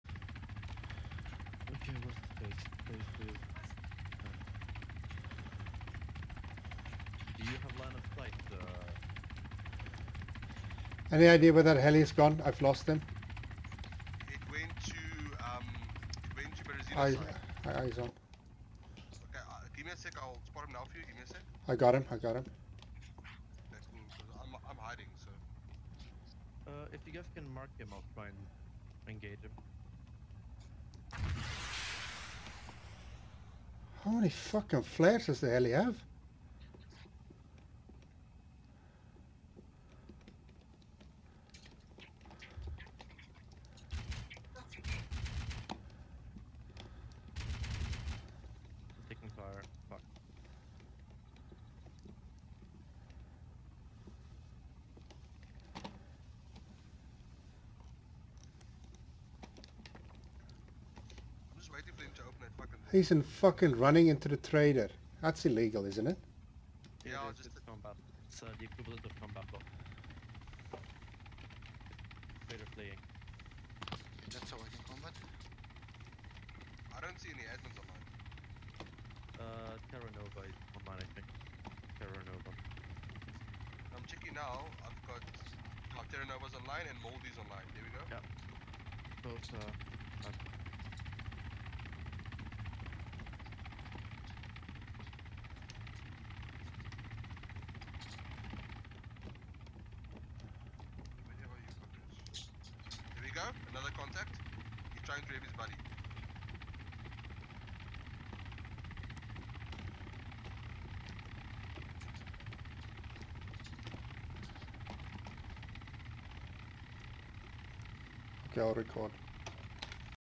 Meistofel combat avoids
Clearly running into the trader to get protection from the safe zone... then I leave him and he gets on my six and kills he.